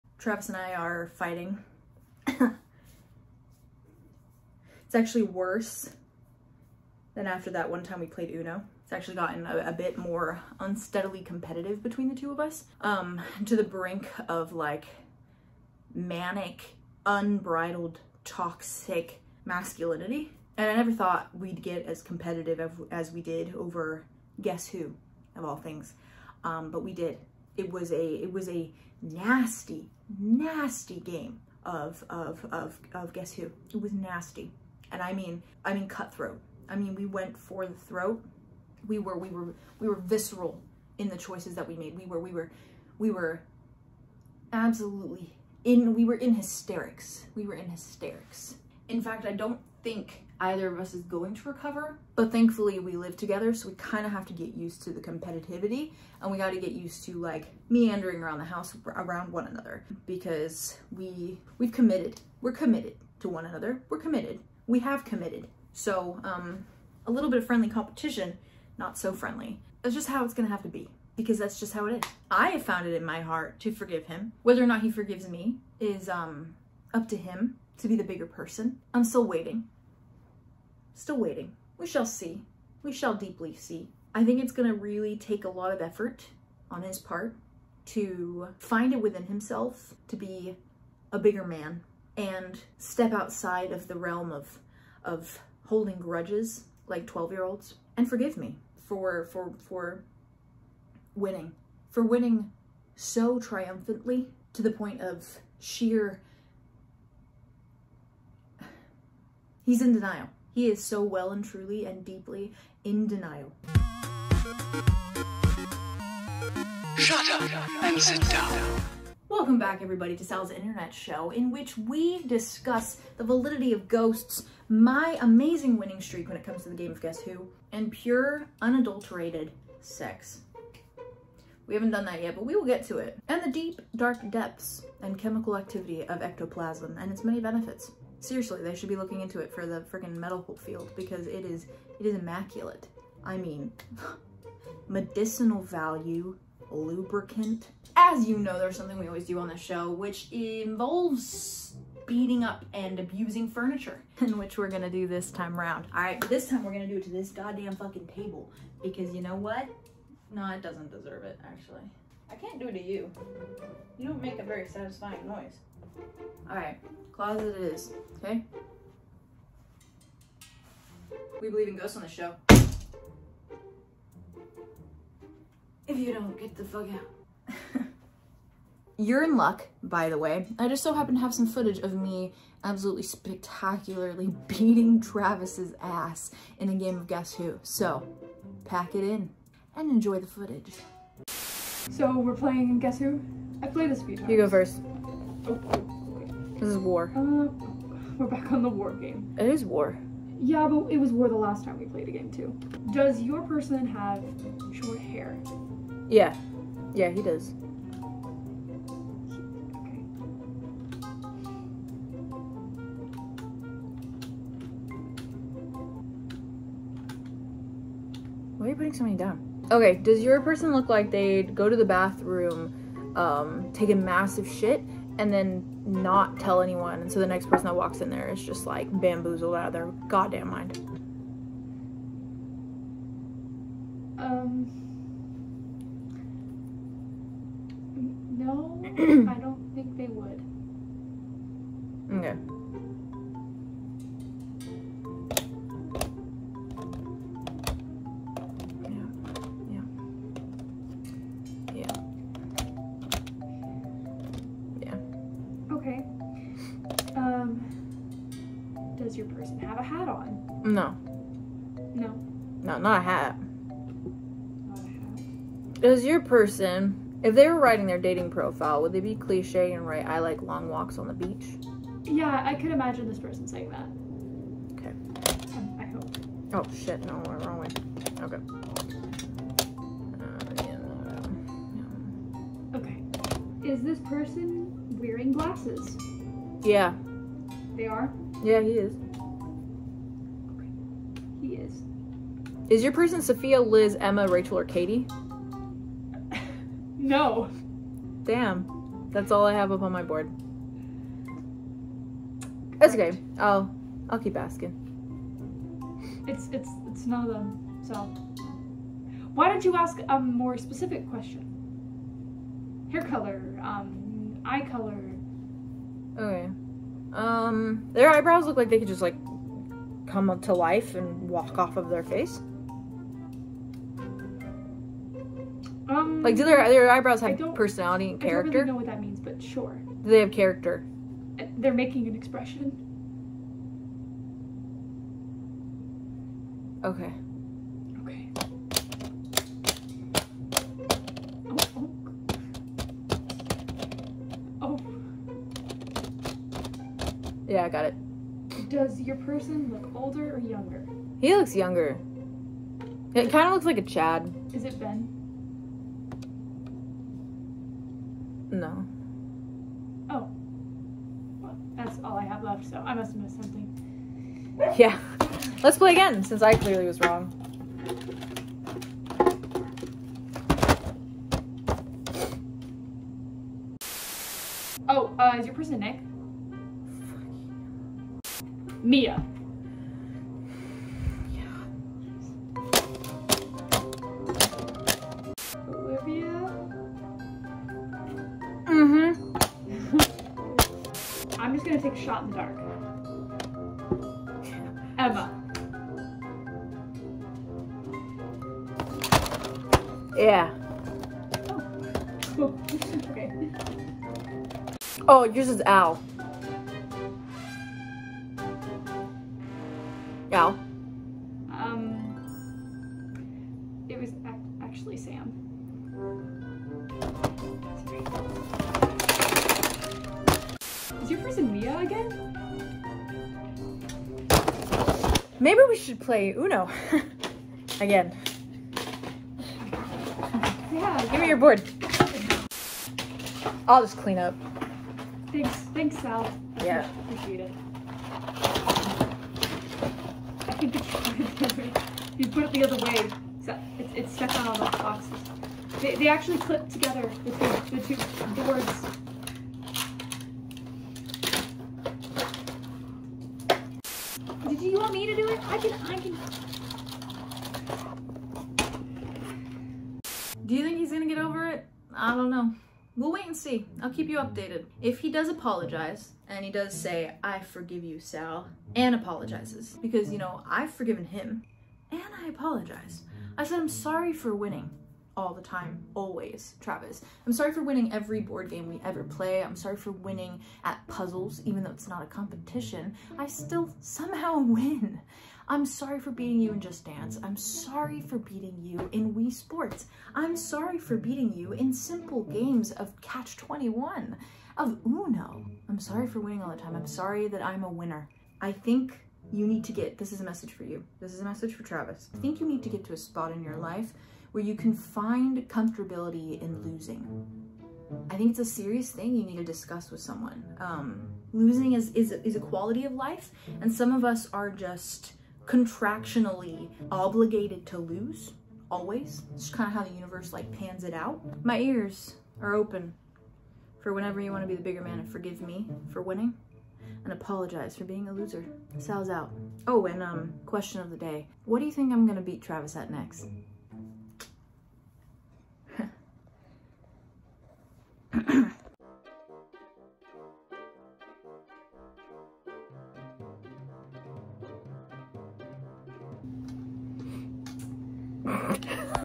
Sal's Internet Show - Season 2 Episode 3 || THE GUESS WHO DILEMMA
The boys are back. And they're fighting???

𝓢𝓸𝓬𝓲𝓪𝓵 𝓛𝓲𝓷𝓴𝓼 & 𝓦𝓪𝔂𝓼 𝓽𝓸 𝓢𝓾𝓹𝓹𝓸𝓻𝓽 𝓤𝓼!

🍄Bof's Etsy Store🍄

💙Bof’s Amazon Wishlist:💙

💚Jazzy’s Amazon Wishlist:💚

🐶Sherlock's Amazon Wishlist:🐶